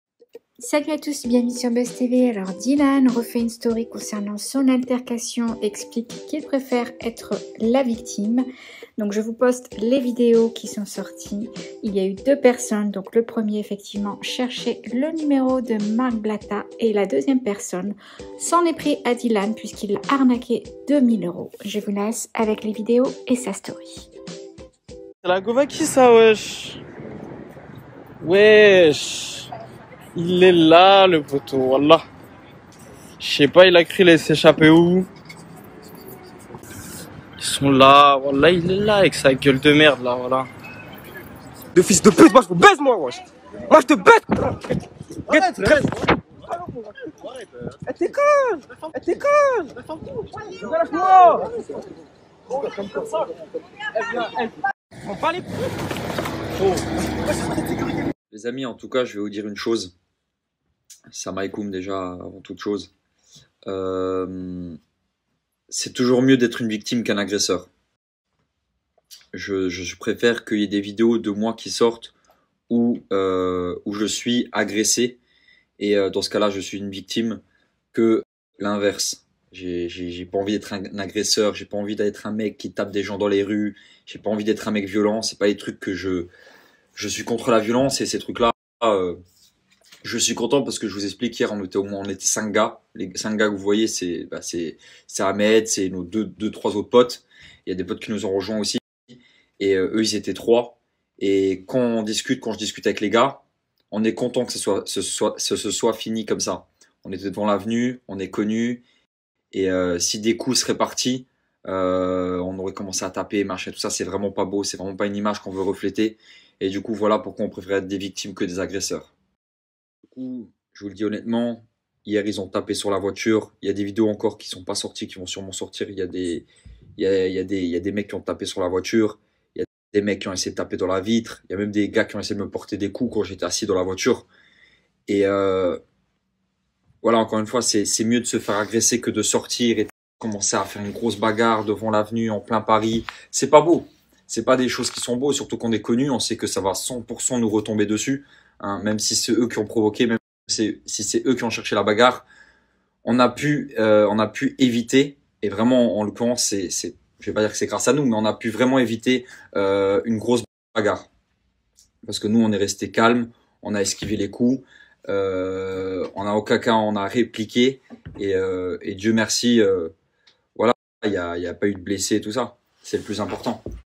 DYLAN A MENTI SUR L' ALTERCATION, 2 MECS L' ATTENDAIENT, '' LA VRAIE VIDÉO!''
Dylan a menti sur les raisons de l'altercation et n'a pas mis les vraies vidéos